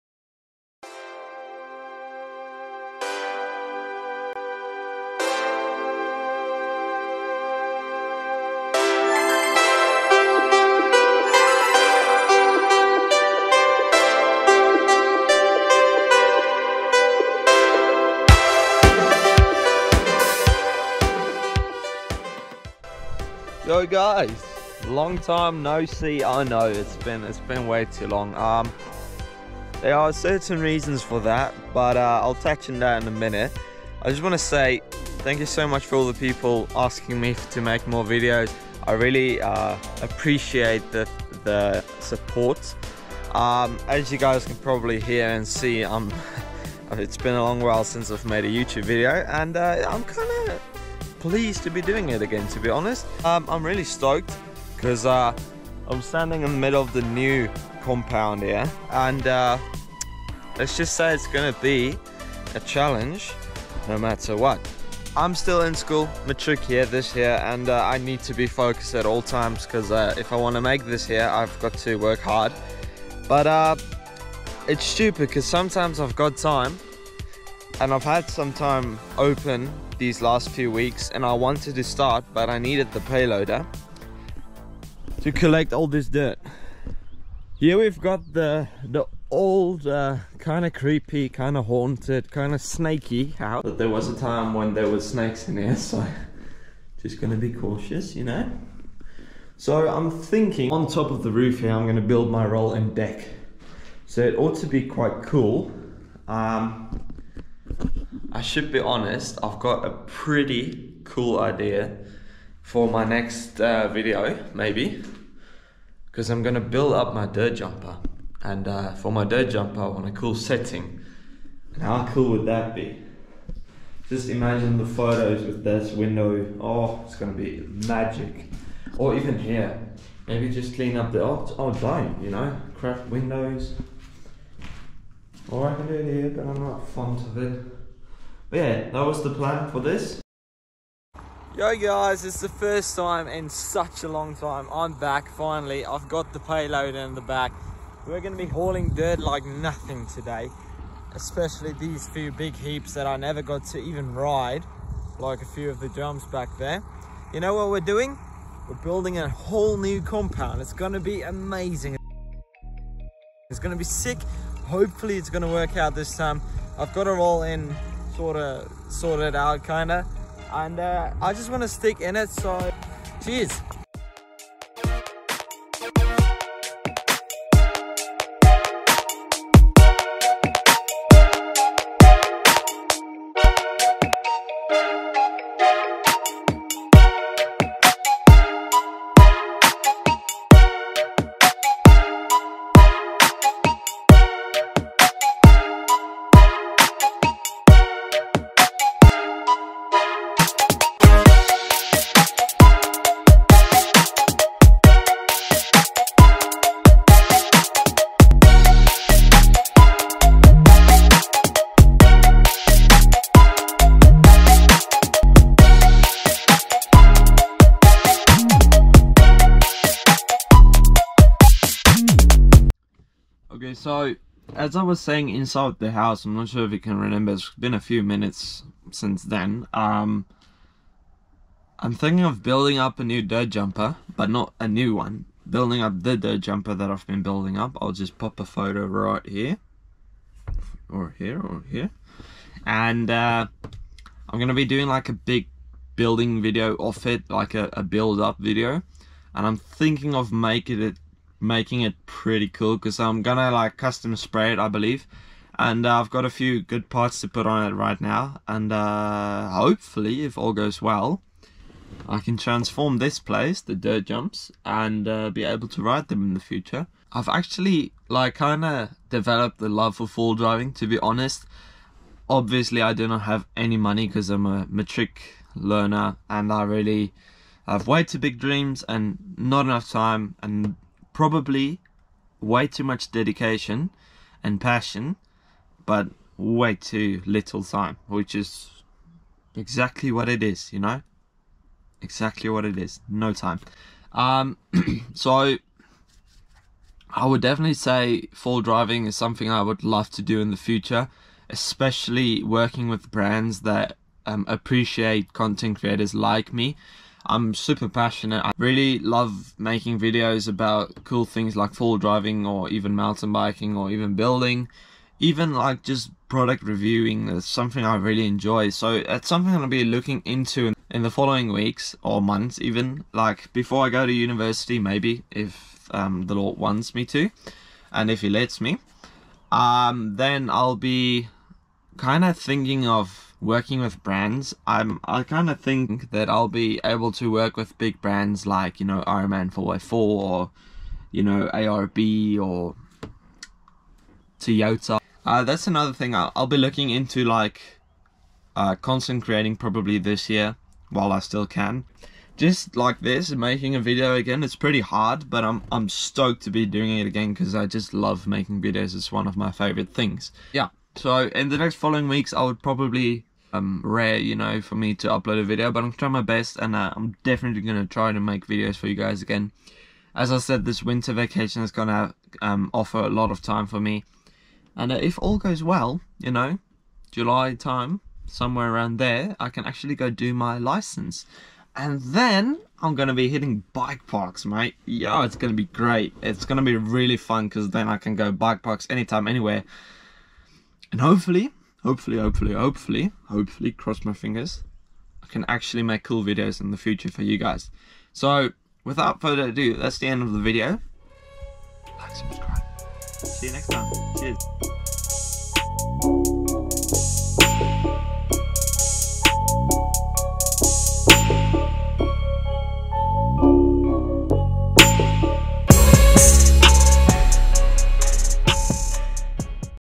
The Fullsend Foundation | Episode 1
Joh guys - hope you’re all doing well - Its a first video in a long time - I know…
But I just had to make an update video to fill you guys in on the newest plans!